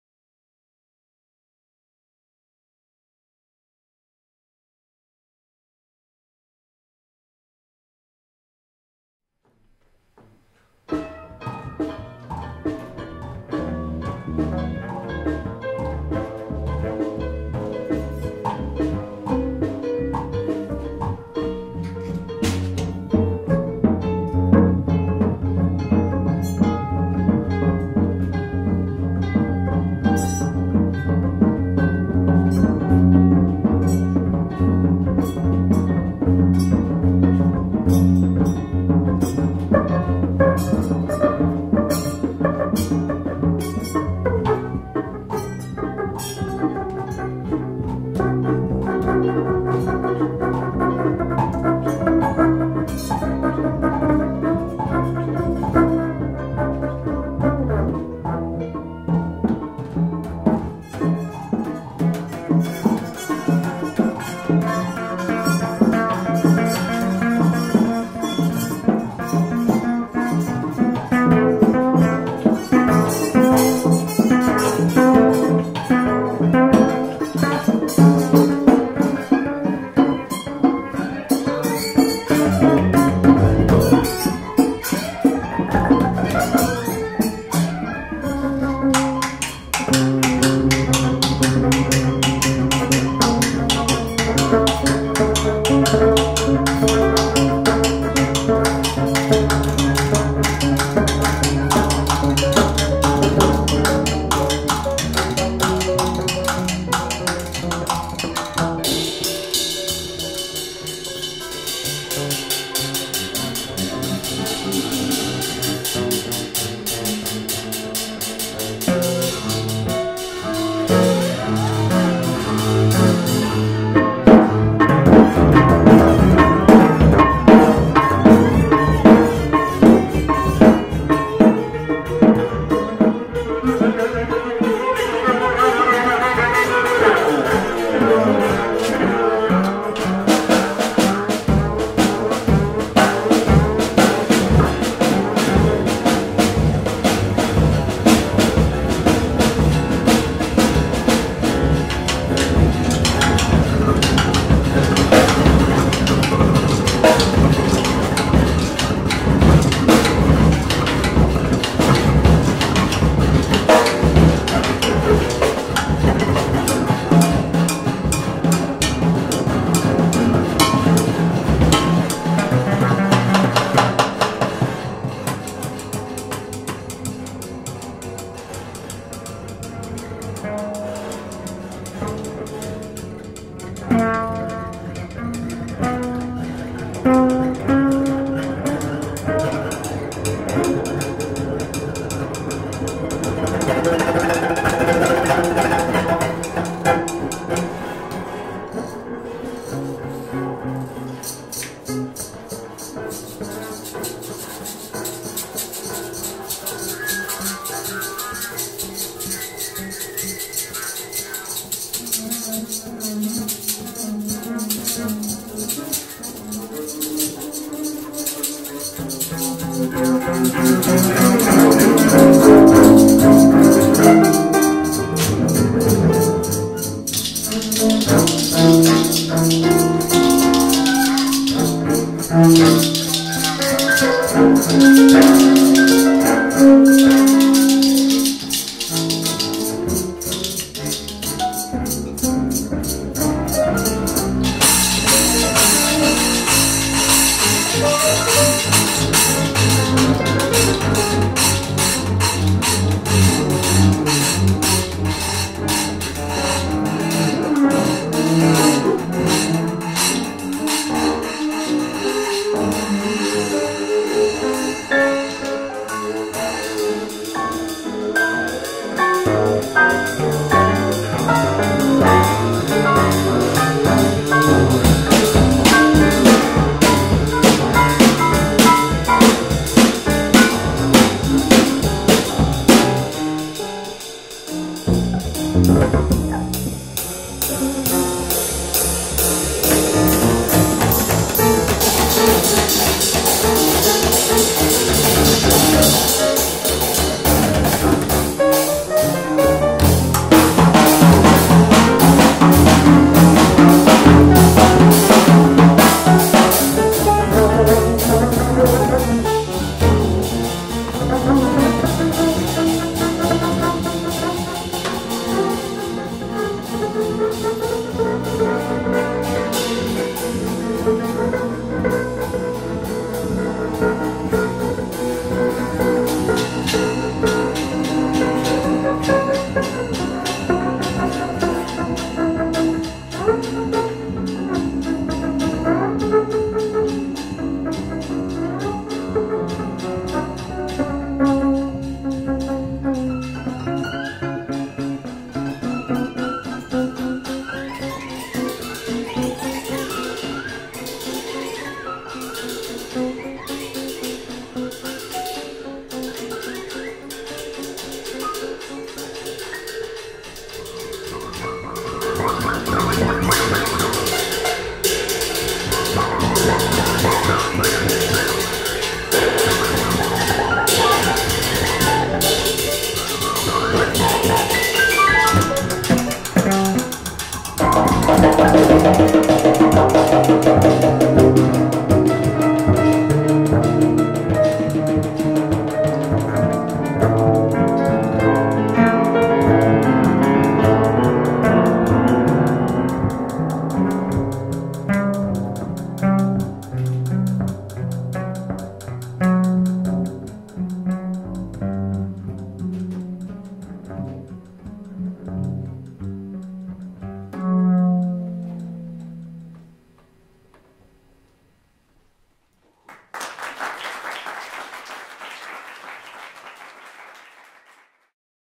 Improv by Denman Maroney (hyperpiano), Mark Dresser (bass), and Michael Sarin (drums)
Recorded and produced by Roulette Intermedium 04/04/04 in concert at the Flea Theater, New York. Thanks to JIm Staley.